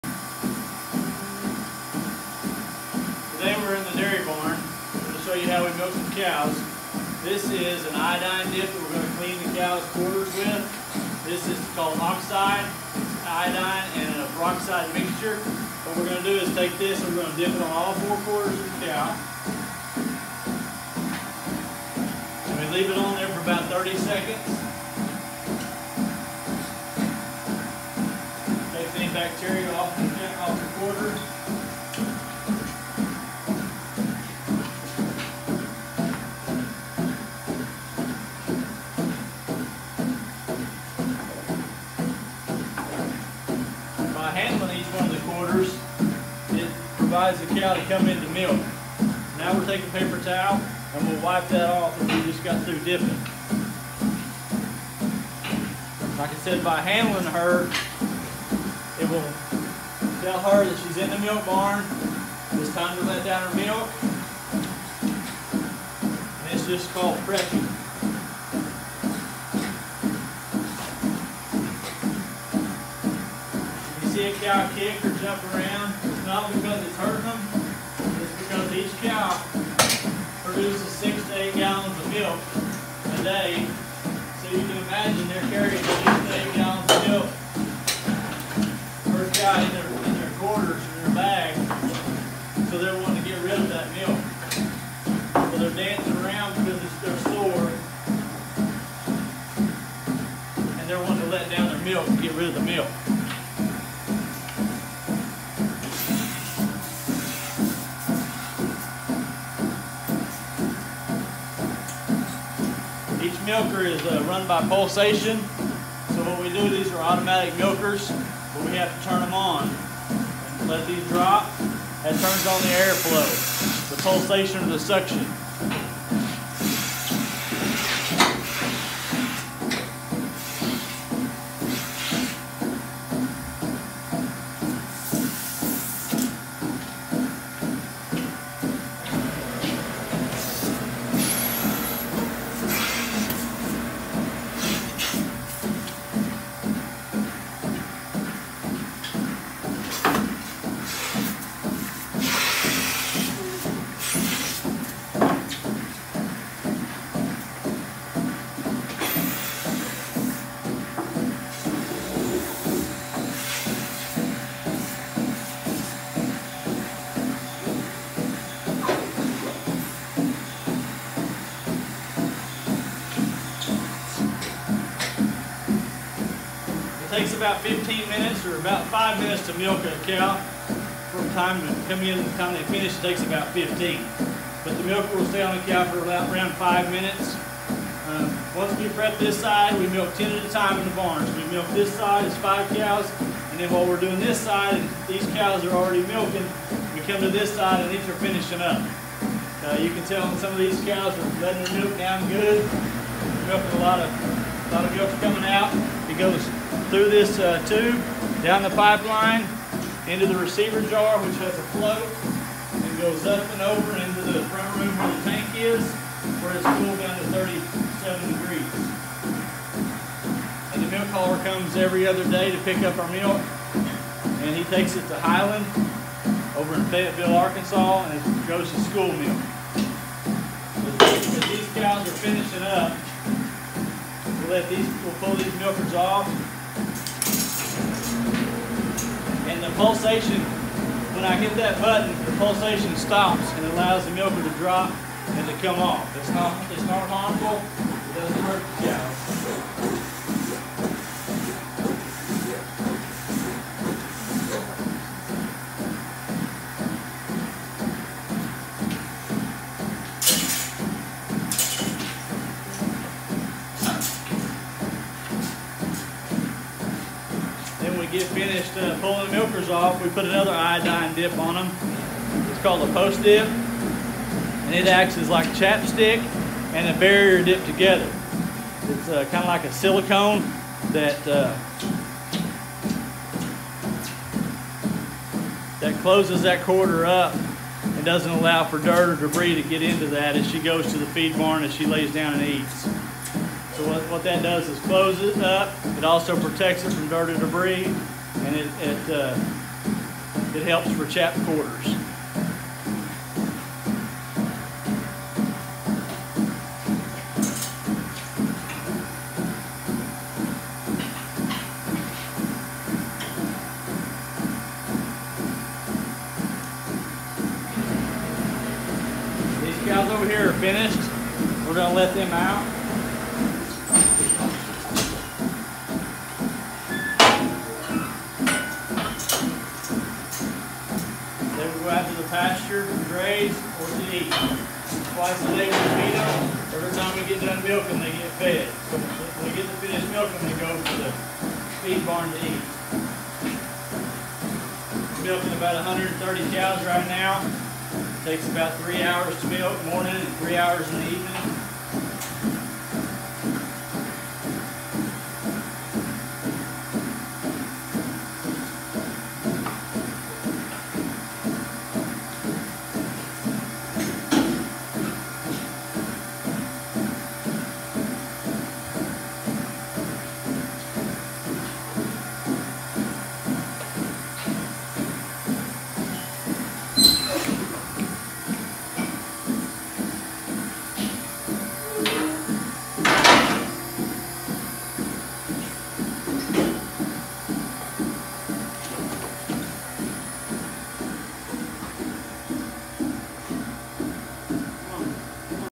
Welcome to Butler Dairy part 2, a few minutes in the milking parlor.
Watch as John milks a set of Holstein cows. He’s milking 5 on each side. We hope you learned something! *this is just an unedited video of milking so don’t expect anything flashy*